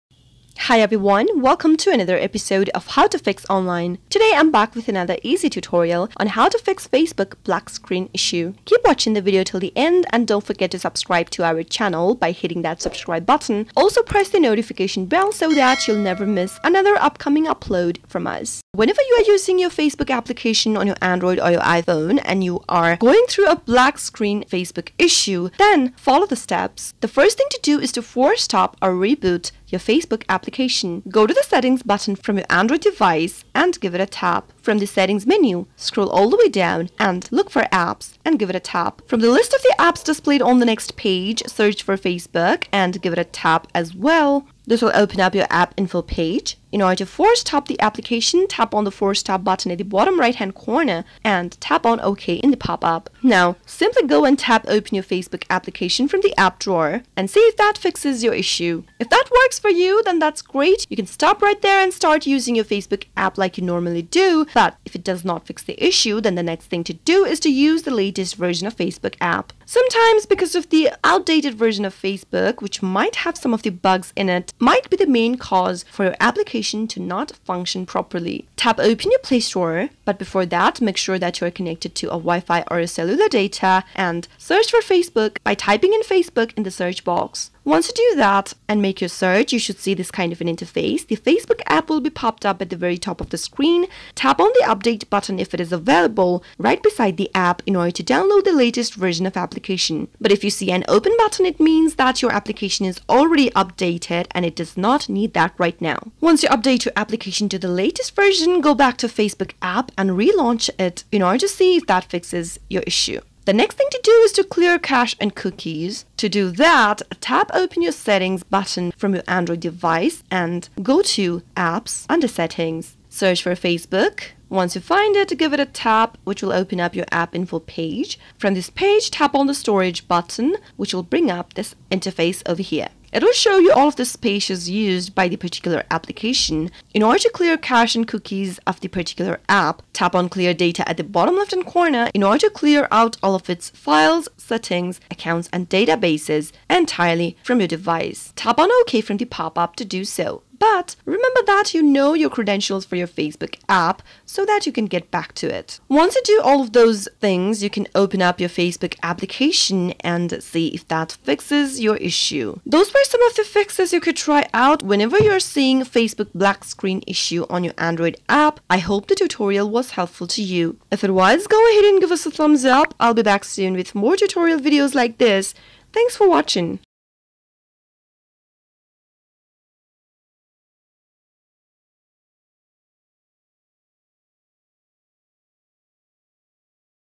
How to Fix Facebook Black Screen 2022?
Having black screen issue on Facebook? Here in this video we will show you step by step guide on how to fix black screen issue on facebook from your mobile device.